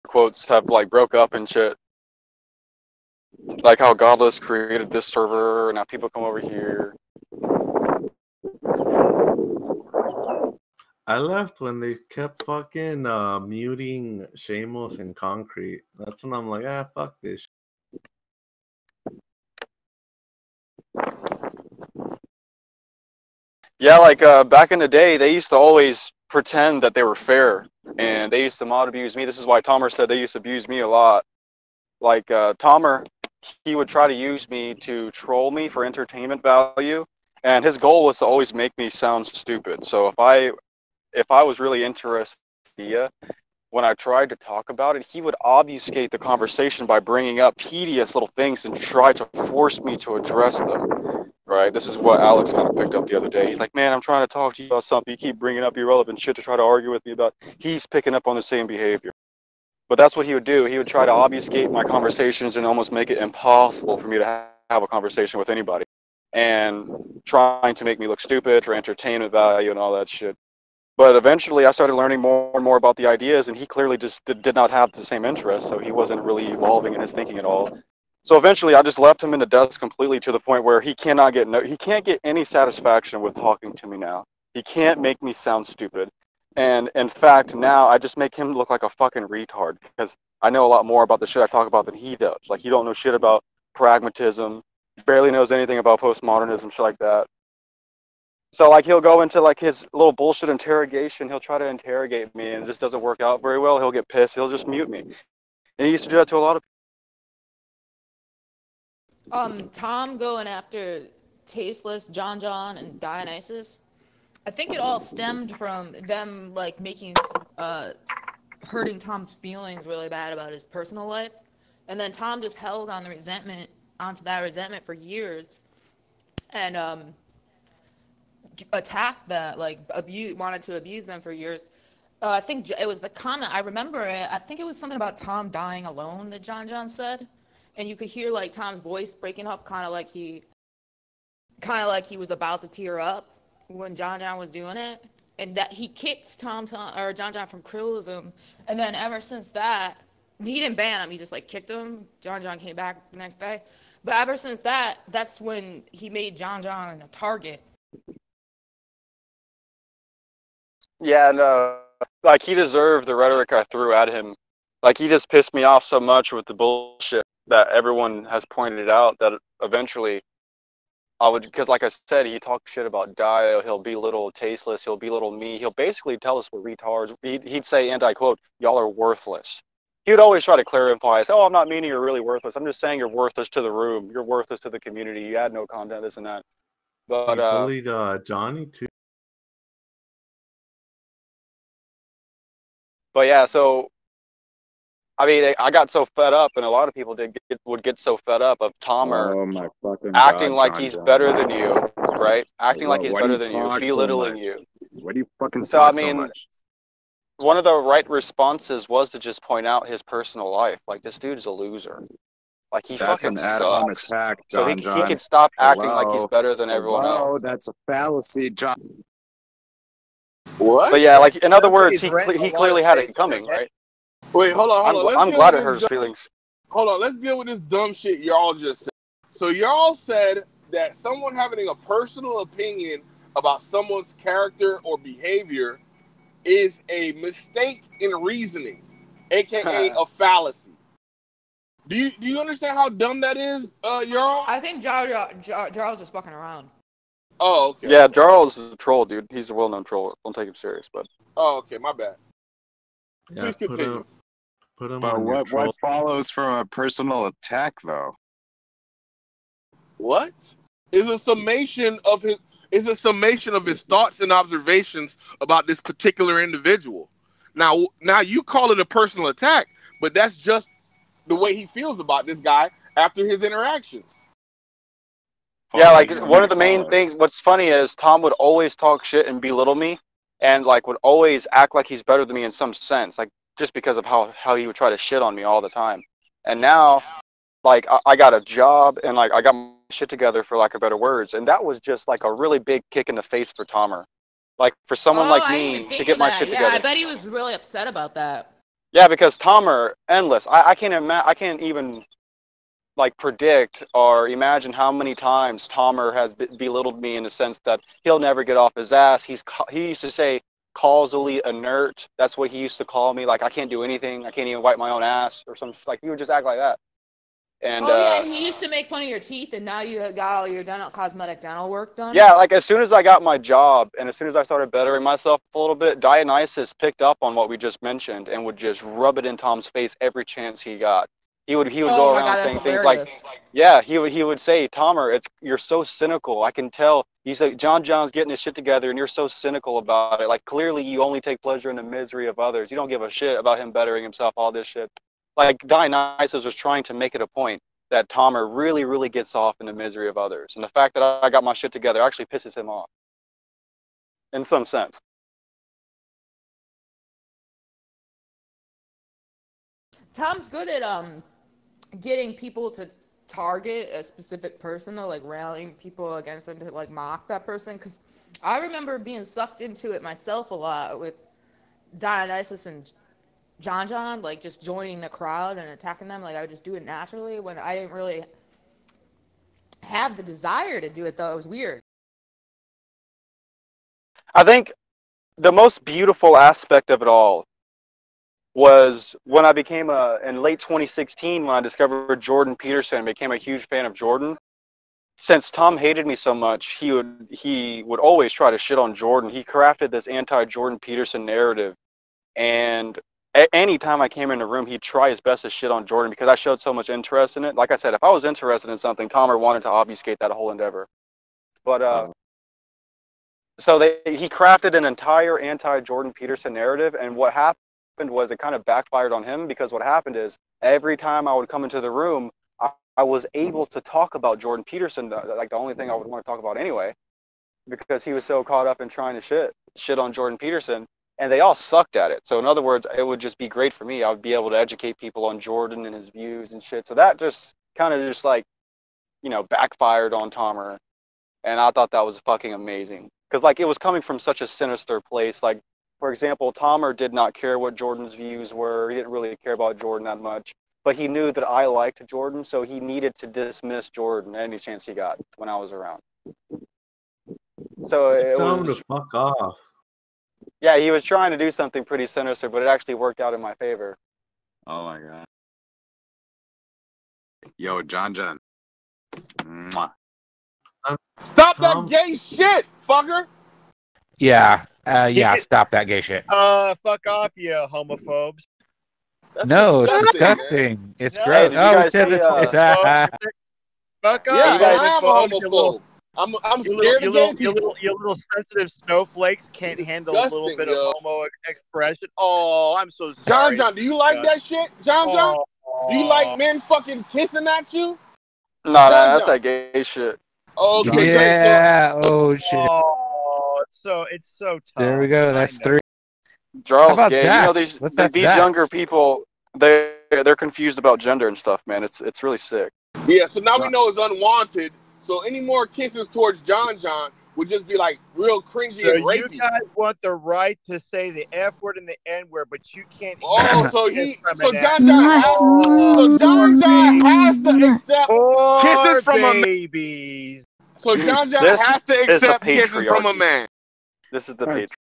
Tom Rabbittt Pitiful Behavior Summarized By JonJon
Jonjon and Godless go over some of the history of Tom Rabbittt and his targeting of Jonjon over the years in the Tom Unboxes Discord Server.

JonJon points out that Tom is upset with how he turned his life around in 2020 and says "Tom with talking to me now he can't make me
sound stupid and in fact now I just make him look like a fucking retard cuz I
know a lot more about the shit I talk about that he does like you don't know
shit about pragmatism"